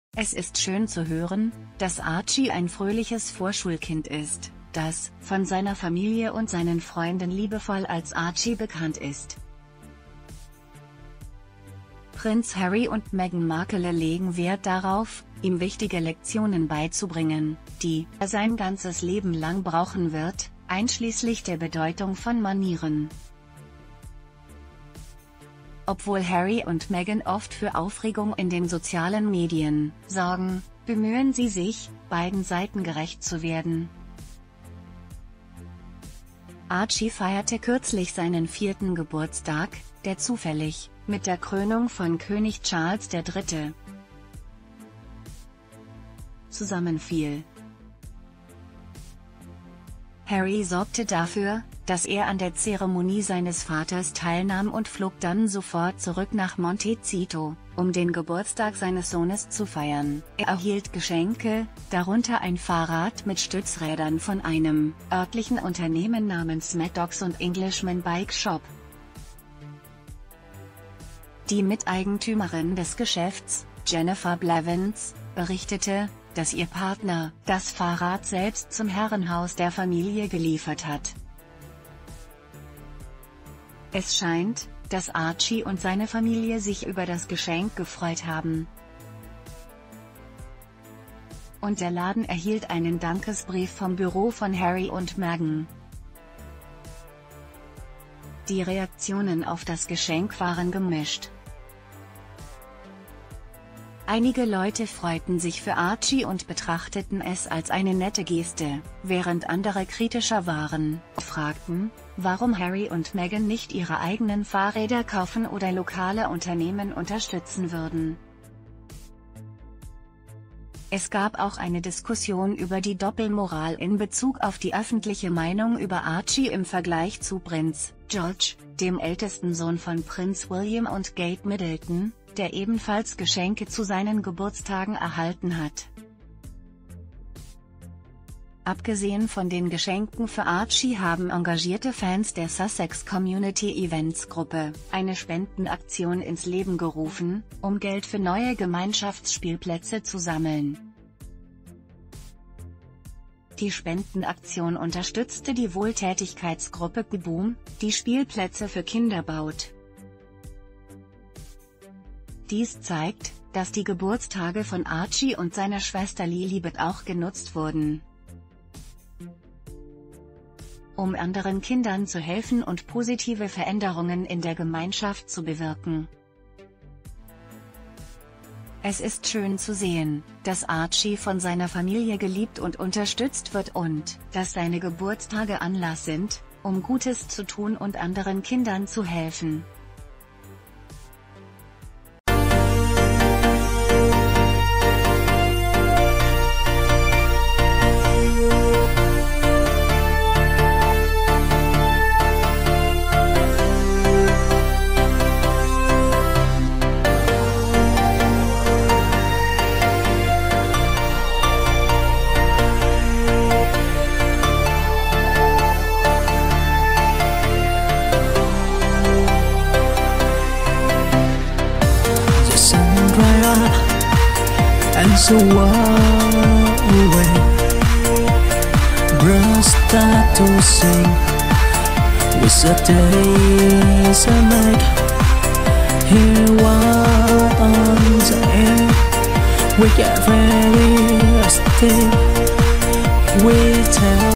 Eines von Archies Geburtstagsgeschenken sorgt für königliche Gegenreaktionen
Offiziell ist er als Prinz Archie von Sussex bekannt, der sechste in der britischen Thronfolge. Für seine Familie und Freunde ist er nur Archie, ein fröhlicher kalifornischer Vorschulkind, der den Geburtstagsfeiern seiner Klassenkameraden frisches Obst mitbringt. Prinz Harry und Meghan Markle, Herzog und Herzogin von Sussex, bringen Archie die wertvollen Lektionen bei, die er sein ganzes Leben lang brauchen wird.